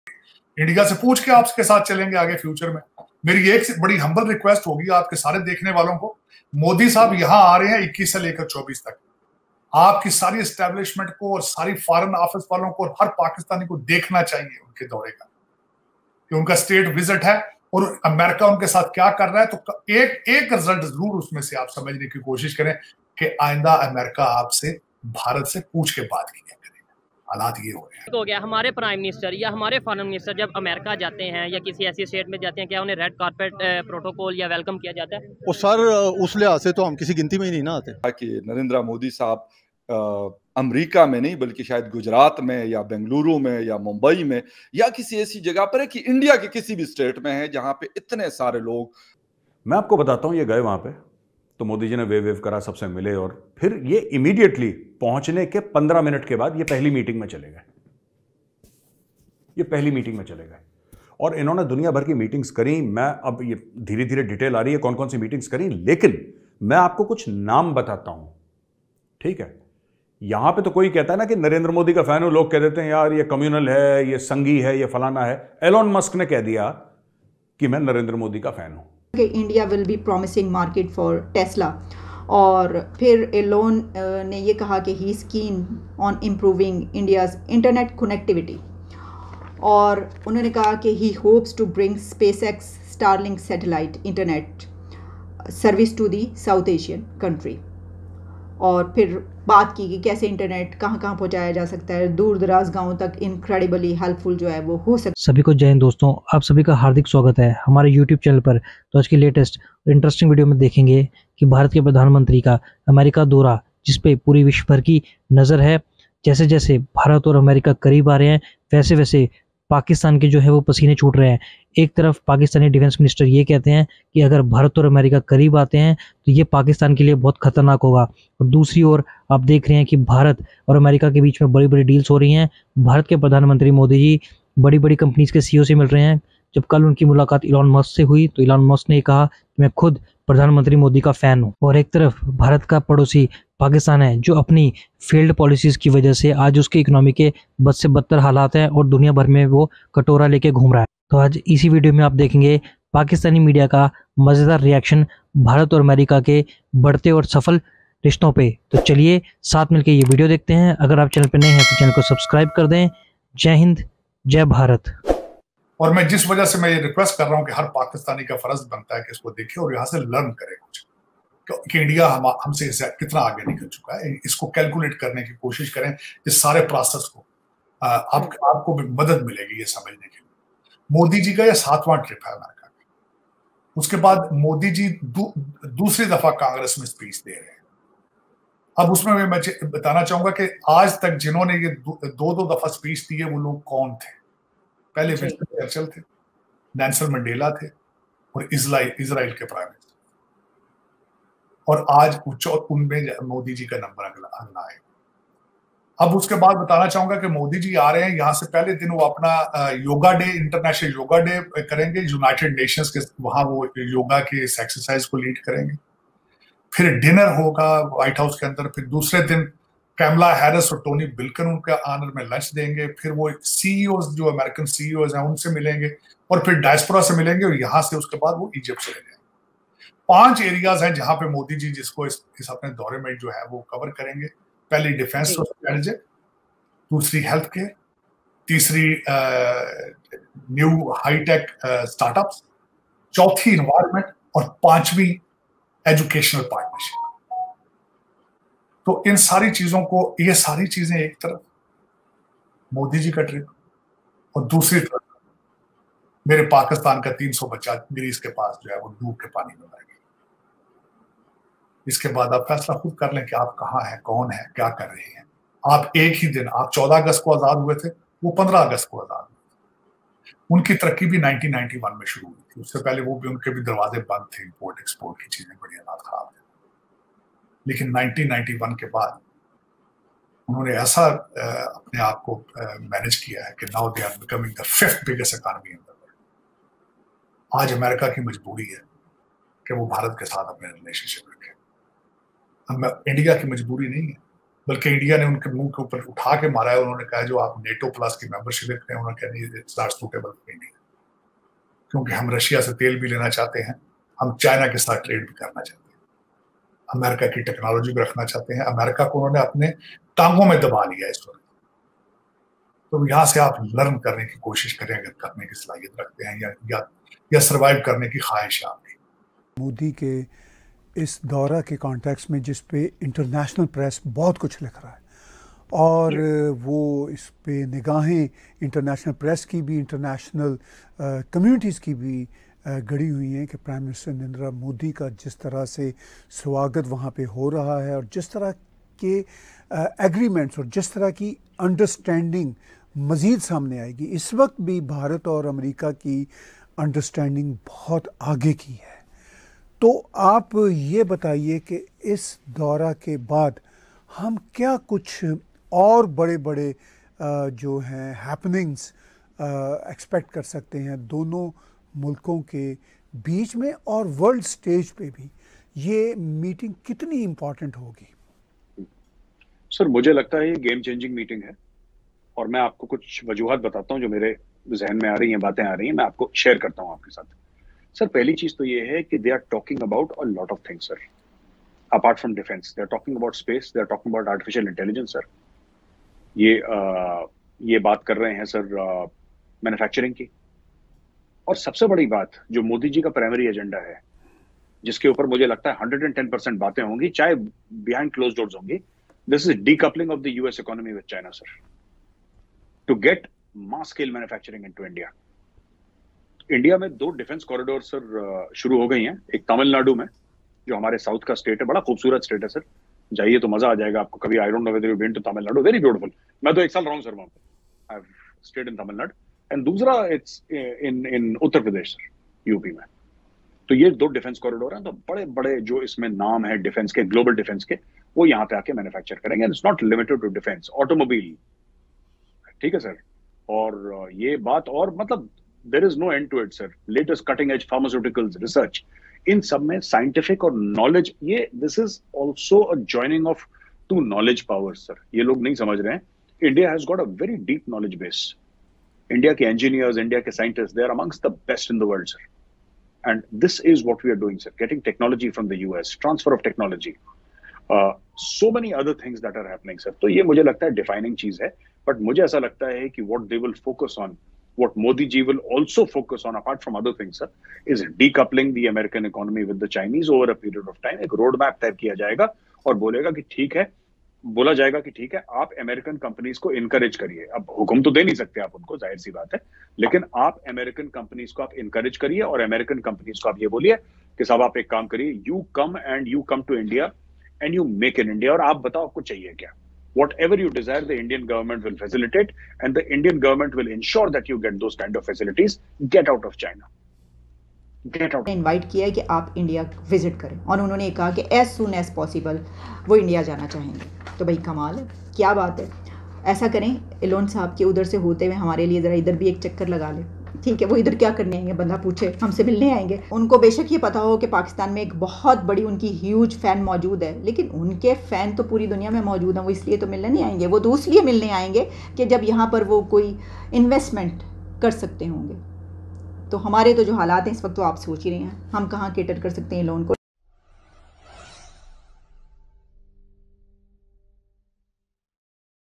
Pak media reaction on Modi creates History in America | Pakistan media on India latest | Modi ins US
Modi ne Racha naya Itihas | Pak media on India latest

Pakistani reaction
pakistan news